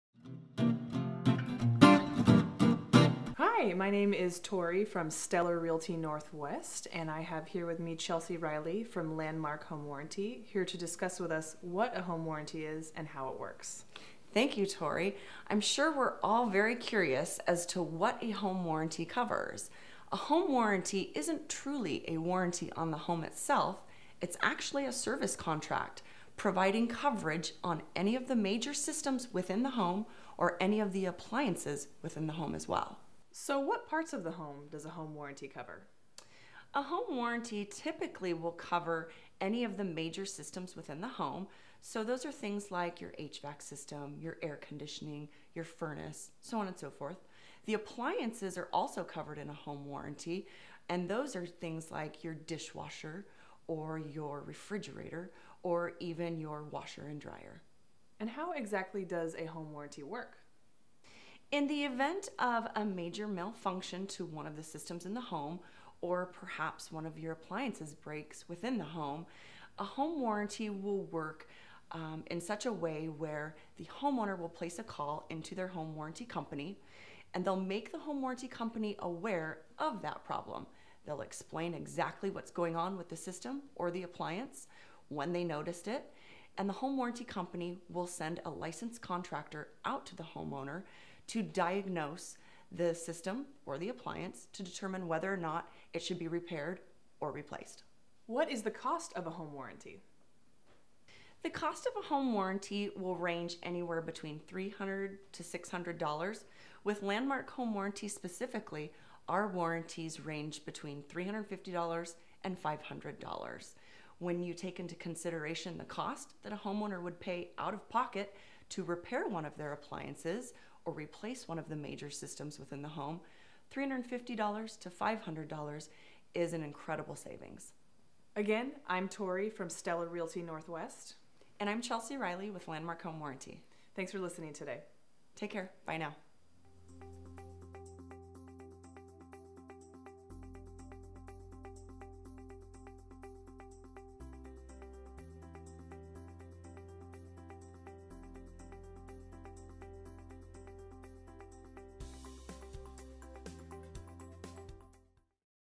Portland Real Estate Agent: Everything You Should Know About Home Warranties
We’ve been getting a lot of questions about home warranties, so we’ve brought our friend Chelsey Reilly of Landmark Home Warranty along to answer them. Although a home warranty doesn’t cover the structure of the home itself, it does cover all the major systems and appliances. These include things like your HVAC system, refrigerator, and your dishwasher. To learn more about home warranties, what they do, and how you can get one, watch this short video.

Stellar Realty NW
Serving Greater Portland & Tri-County Areas

TRANSCRIPTION:

We’re delighted to be joined today by Chelsey Reilly of Landmark Home Warranty to talk a little bit about home warranties, what they cover, and how you can get one if you don’t have one already.

A home warranty isn’t a true warranty on the home itself, it’s actually a service contract providing coverage on major systems in the home and appliances within the home. It covers the major systems in your home, like your HVAC, your plumbing, as well as appliances like your dishwasher, refrigerator, and washer/dryer.

In the event of a major malfunction to one of the systems or appliances in the home, a home warranty will allow a homeowner to place a call to their warranty company and make them aware of the problem. You will explain everything you know about the problem, then they will send a licensed contractor out there to diagnose the system or appliance and determine whether or not it should be repaired or replaced.

The cost of a home warranty will range anywhere from $300 to $600. Landmark Home Warranty has rates between $300 and $500. When you consider the out of pocket expenses for repairing a major system or replacing an appliance, the $300 to $500 bill is an incredible savings.